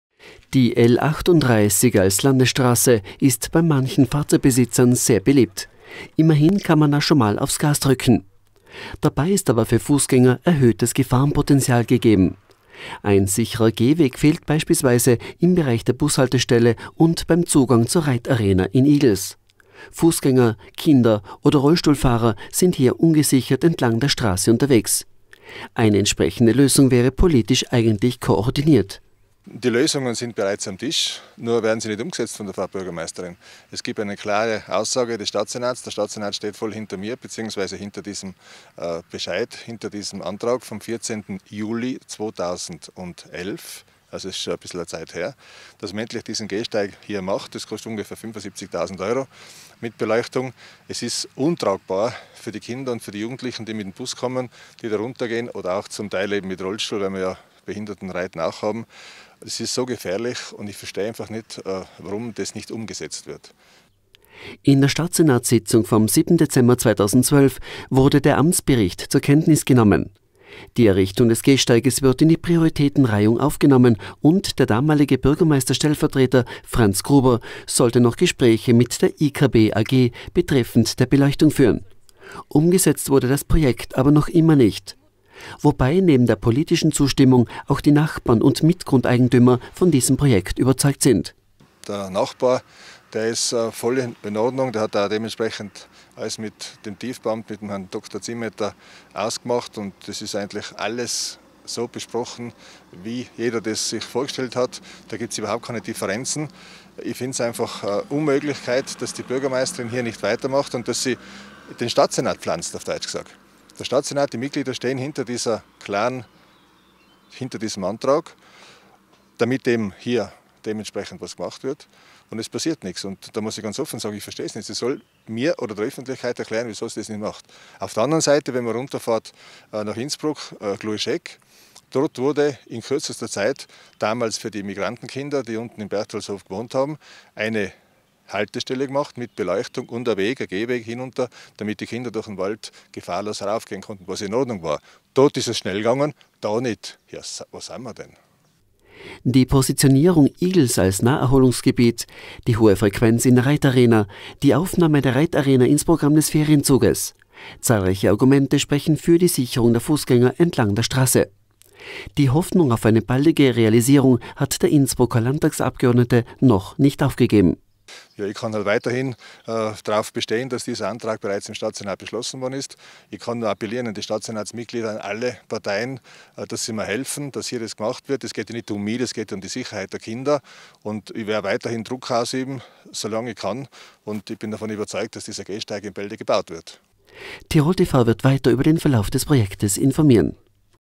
Gehwegprojekt Reit Arena Igls;
Eigentlich ist politisch alles auf Schiene für den geplanten Gehwweg zwischen der IVB Bushaltestelle und der Reit Arena in Igls (Gemeinderatsantrag 2011, Stadtsenatsbeschluß 2012) , nur den Gehweg gibt es immer noch nicht; (C) 2013